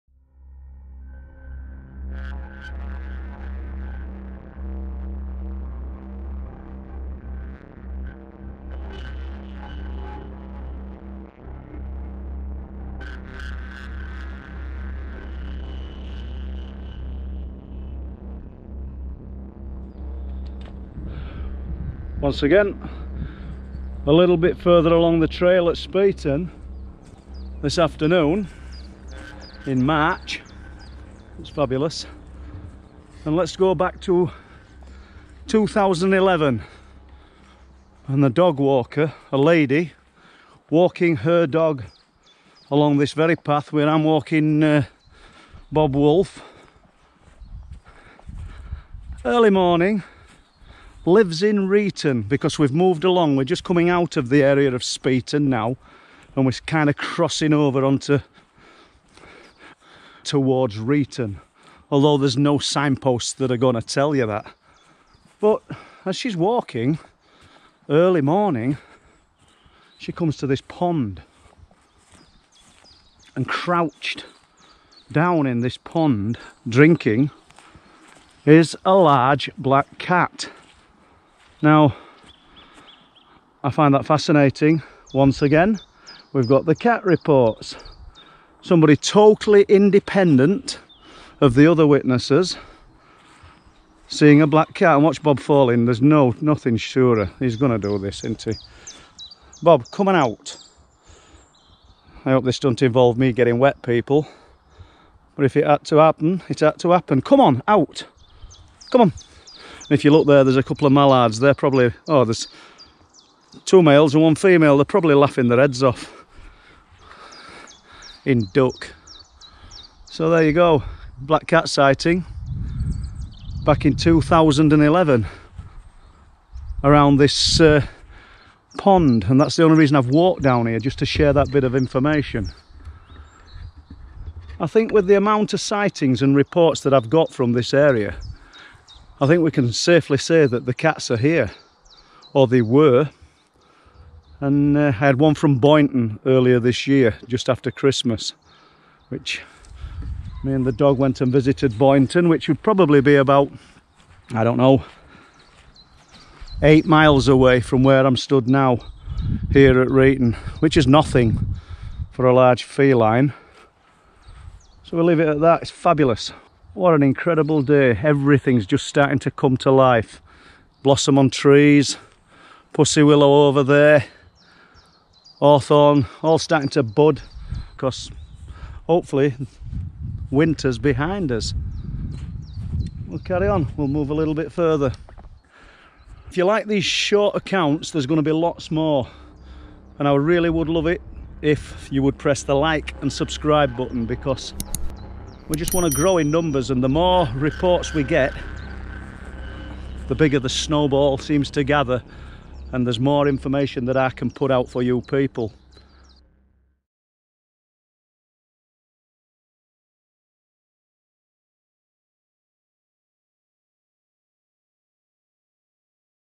Truth Proof - The Crouching Panther
A dog walker in East Yorkshire come's across a crouching black panther.
Paul Sinclair shares his accounts of unusual and strange happenings in an around East and North Yorkshire.